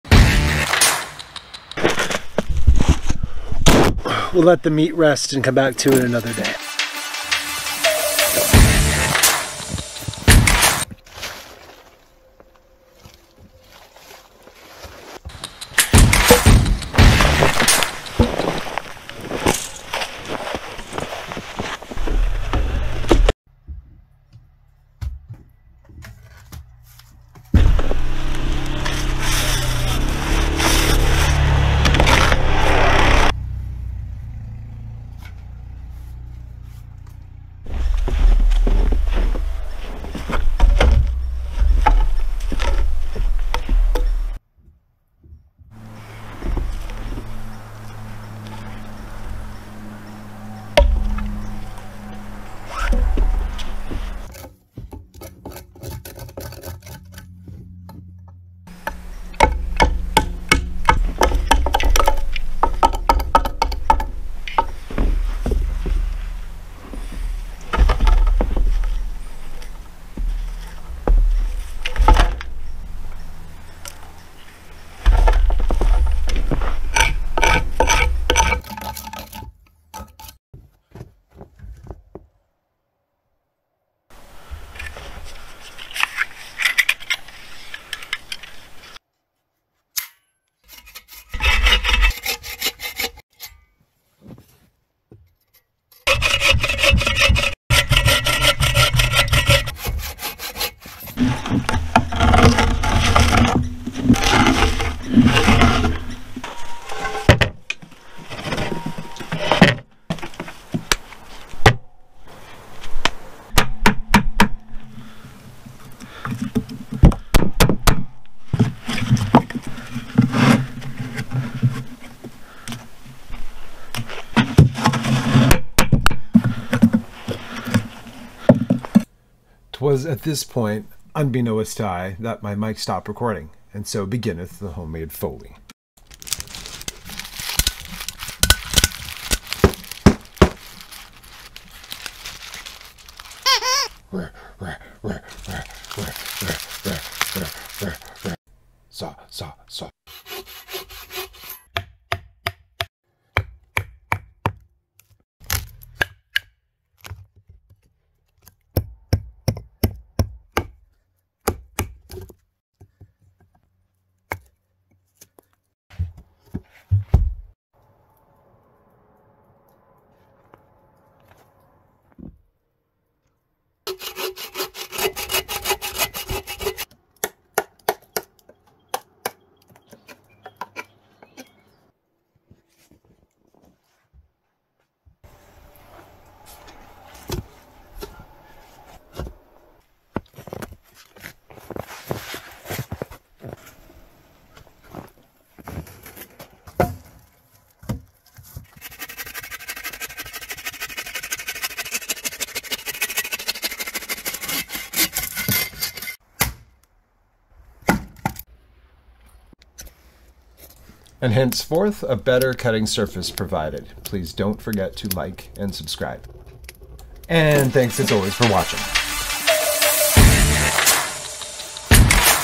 How To Build a Sawbuck DIY
Last bit of snowy meditation before the spring rains - deep trance this learning episode while you watch me struggle with mic failures and elapsed daylight in this short saga of bark, hatchet and saw.

Cut wood off the ground and process it for winter and other storage with ease!